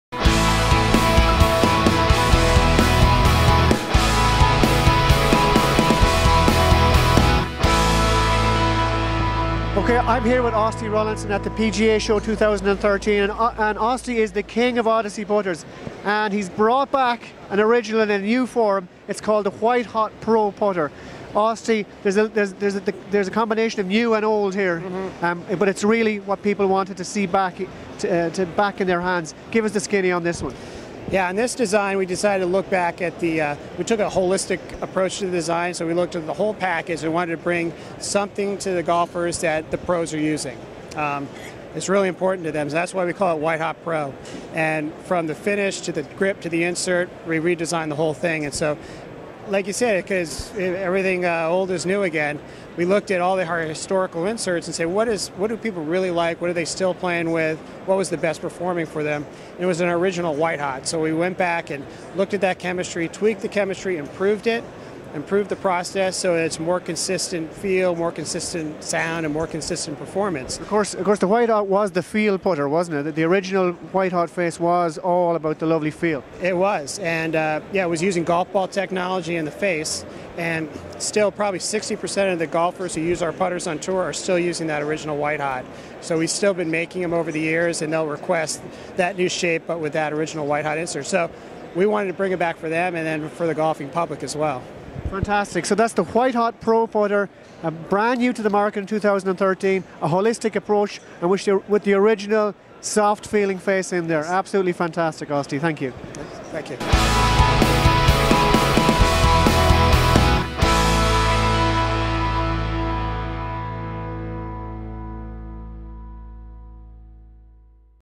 Odyssey White Hot Pro Putter / Review, Features and Benefits / 2013 PGA Show Demo Day
Austie Rollinson, Principal Designer for Odyssey takes us through the new Odyssey Versa putter while on the putting green at the 2013 PGA Show in Orlando. He is the man behind the designs so who better to talk to about its amazing story and technology and who's playing it on Tour.